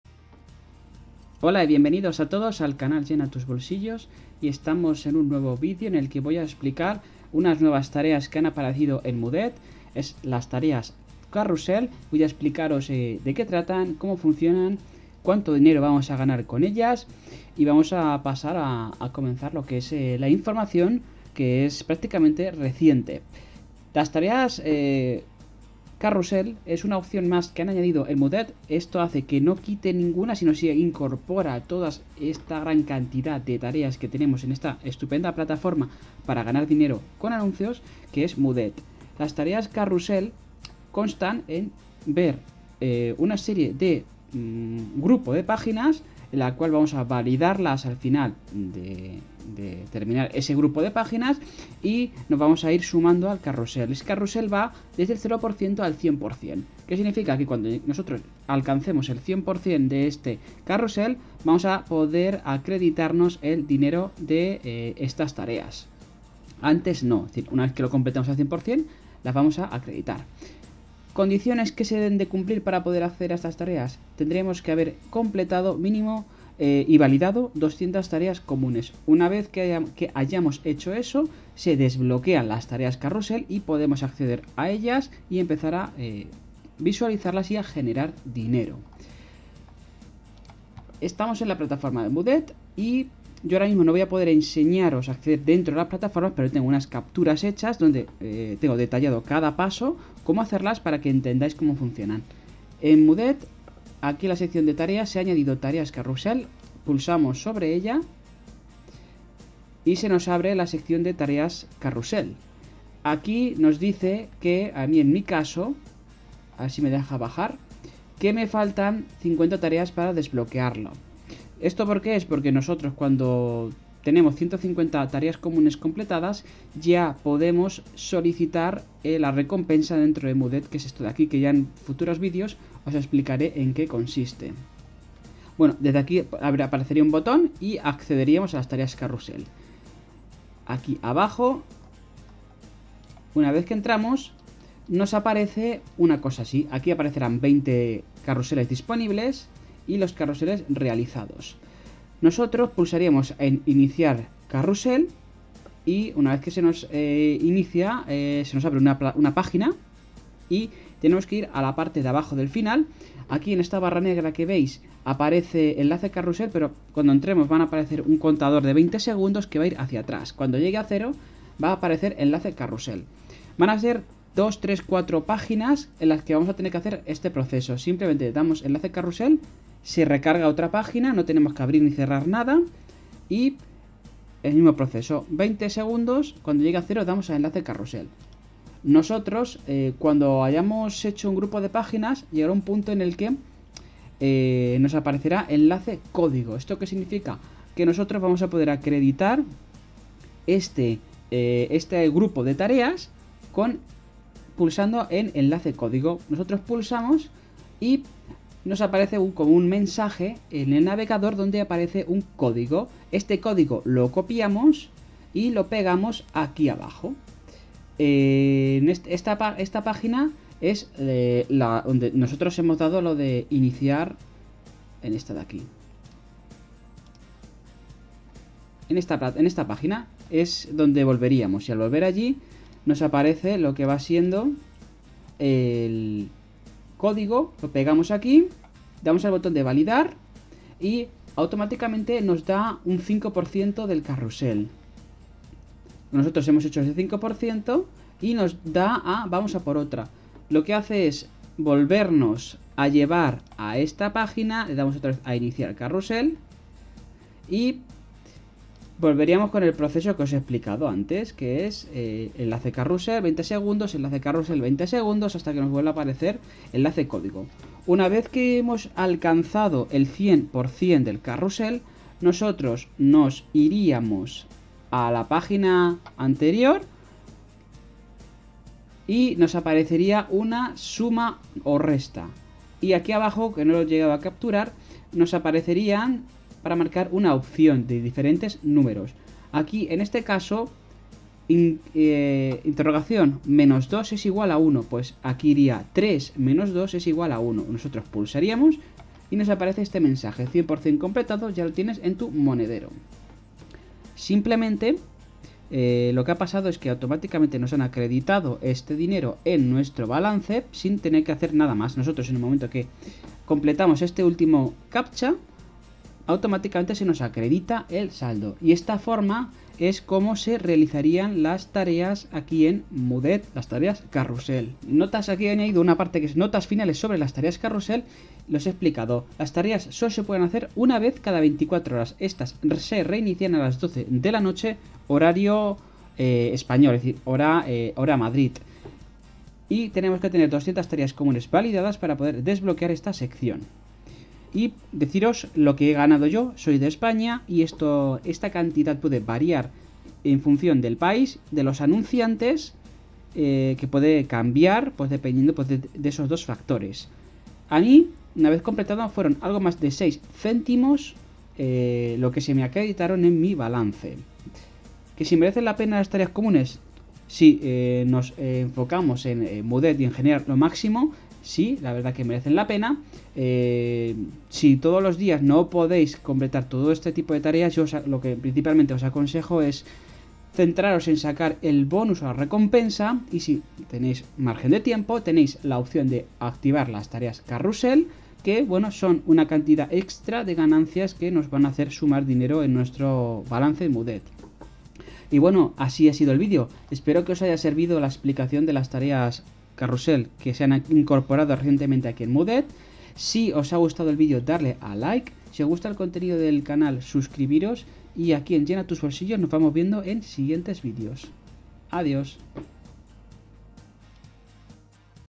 Ya no estoy en Mudet - Como funcionan las tareas Carrsuel | Tutorial Mudet en Español
🔴  NUEVO VÍDEO   🔴

Como funcionan las tareas Carrsuel | Tutorial Mudet en Español

Nuevo vídeo tutorial de Mudet donde seguimos explicando cada uno de los apartados que tiene este negocio para entender como podemos ganar mucho dinero gratis.

Hablamos de las TareasCarrusel que es otra forma más de generar ingresos y que no hace más que sumar formas de hacer dinero en esta estupenda página.